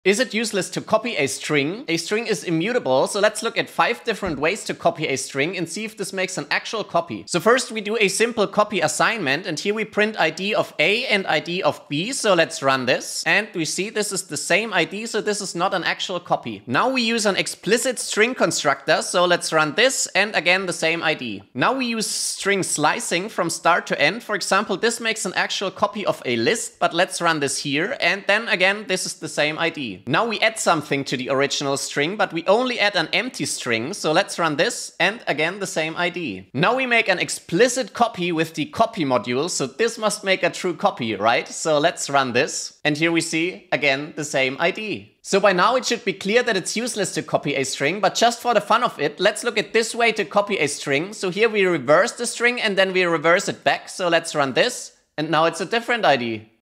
Is it useless to copy a String in Python?? (5 Options)
A string is immutable, so let's look at 5 different ways to copy a string and see if it creates an actual copy.

CONNECT

SUPPORT ME